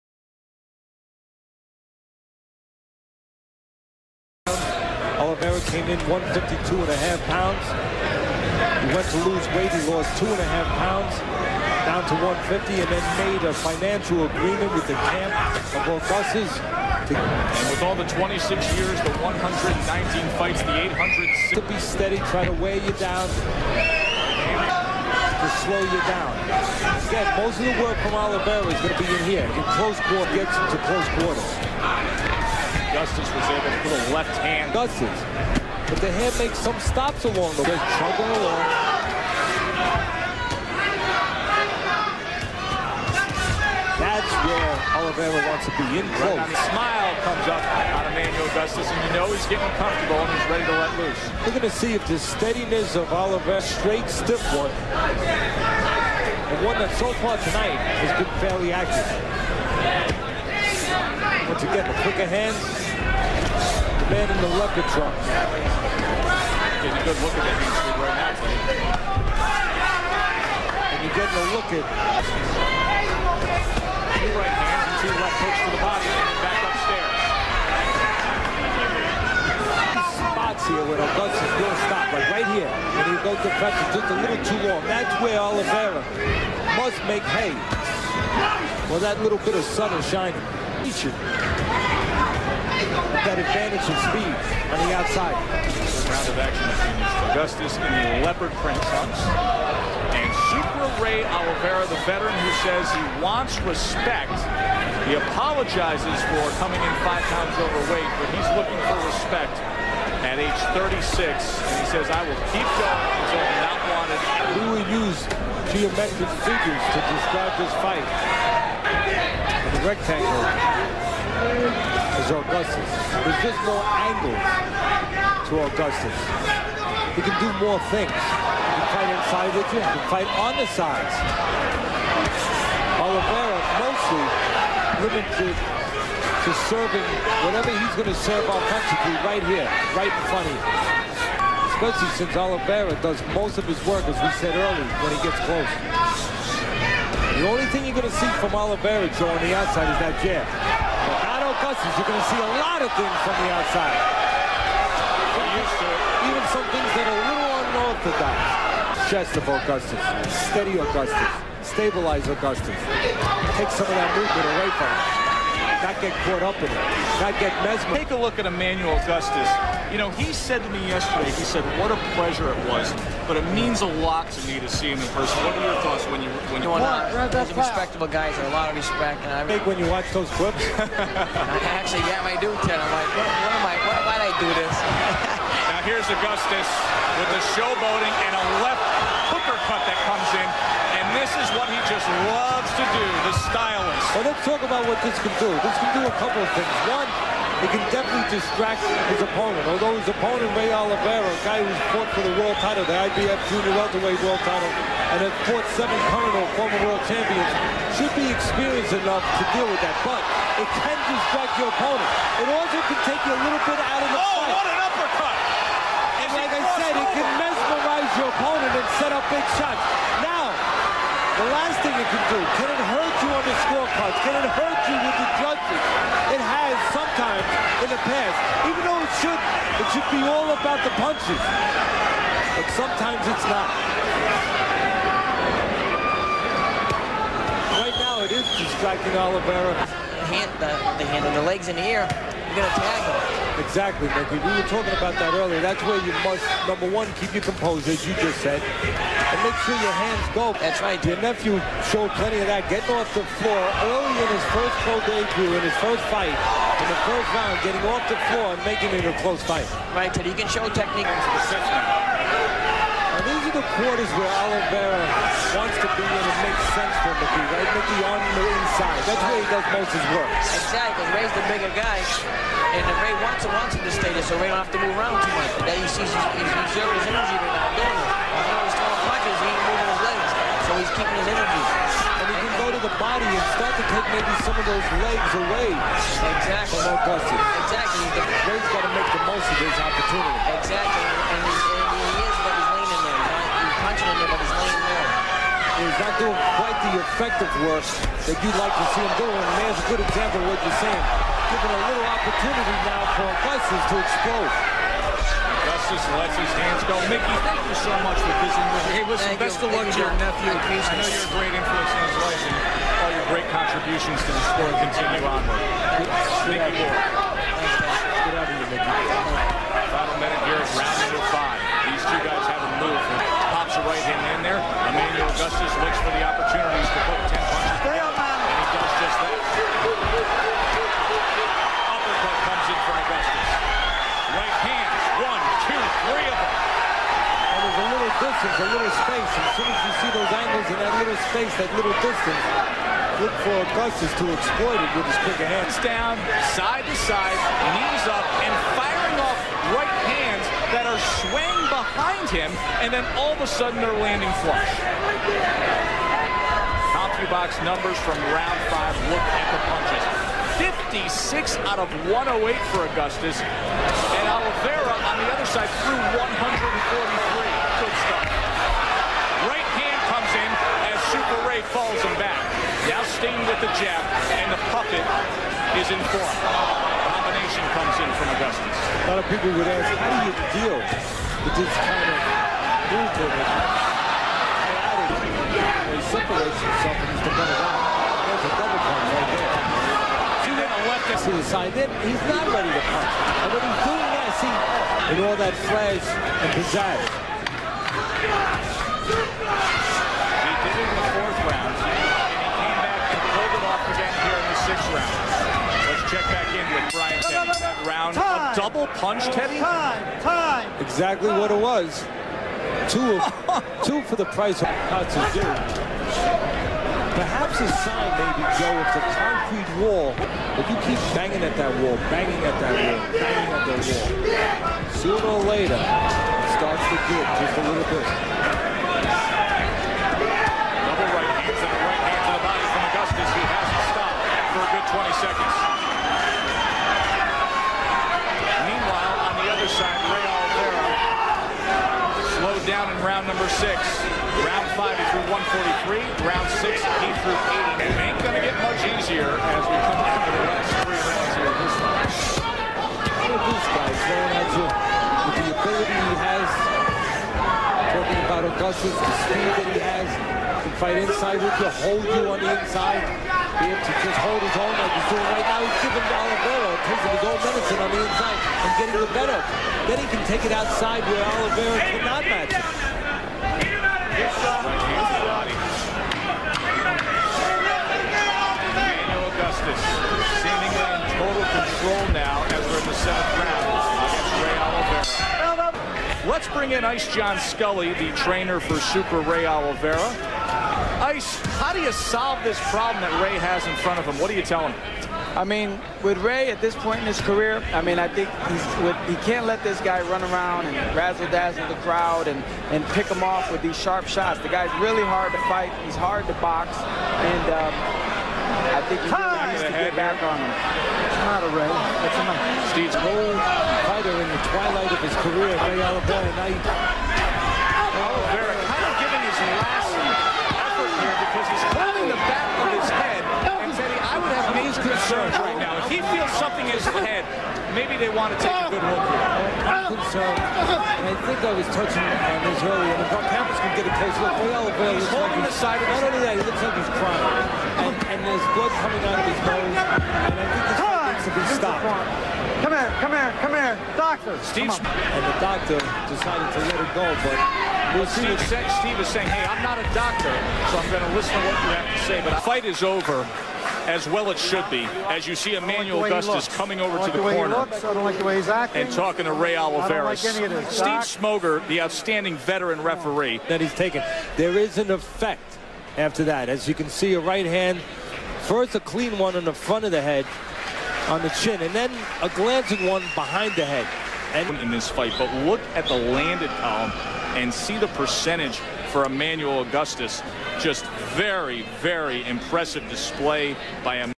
Emmanuel Augustus vs Ray Oliveira - Highlights (Augustus DOMINATES Oliveira)
boxing slug emmanuelaugustus thedrunkenmaster legends augustusvsolliveira epic Why not try watch some boxing highlights here mate ??